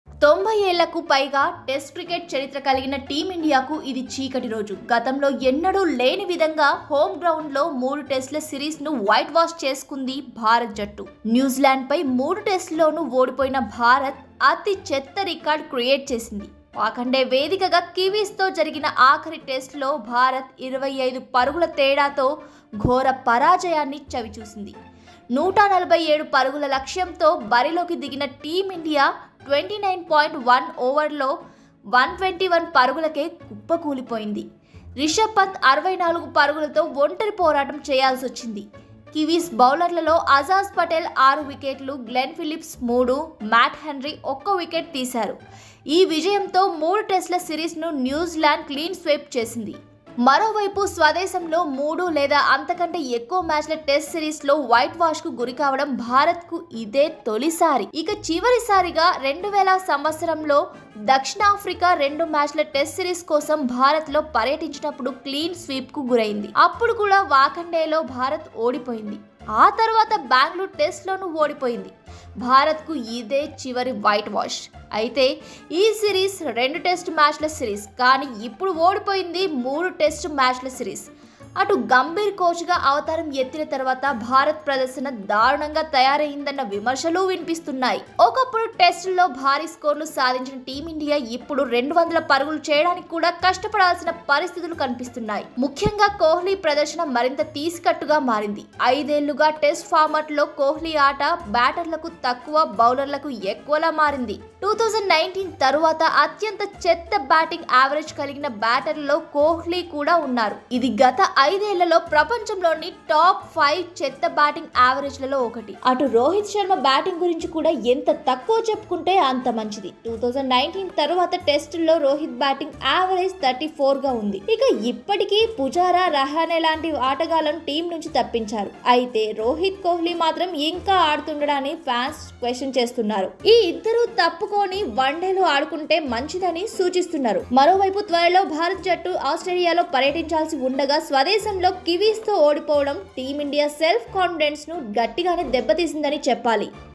New Zealand Whitewash India in Test Match | Virat Kohli | Rohit Sharma | Gautam Gambhir
90 ఏళ్ల క్రికెట్ చరిత్రలో
భారత్‌కు చీకటి రోజు..